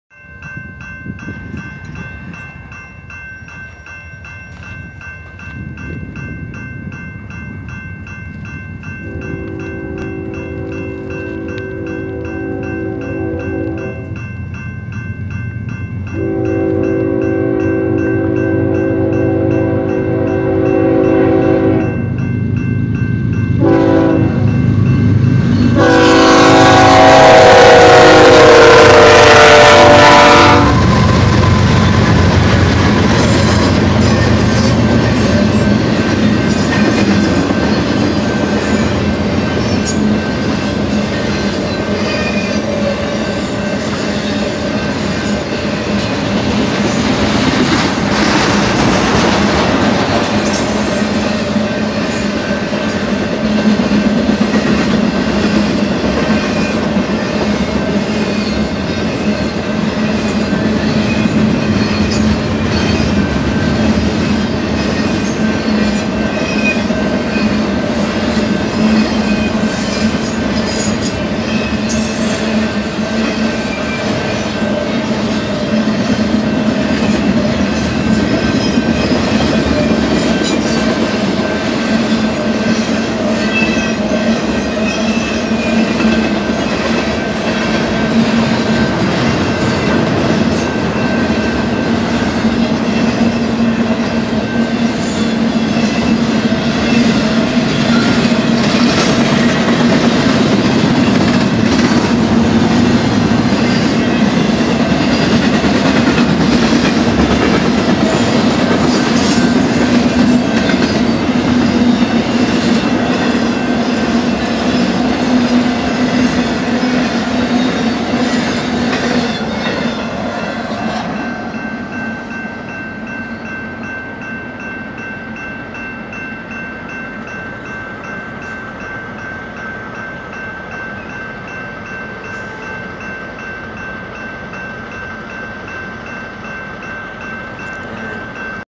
EB BNSF Spine Cars/Intermodal In Daggett Ca!
A eastbound BNSF spine cars/intermodal went through here in Daggett Ca!

Front Engines
8184-ES44C4 (BNSF) (On The Point)
8007-ES44C4 (BNSF) (Trailing)
5251-Dash-9C44-9W (BNSF) (Trailing)
4038-Dash-9C44-9W (BNSF) (Trailing)
8116-ES44C4 (BNSF) (Trailing)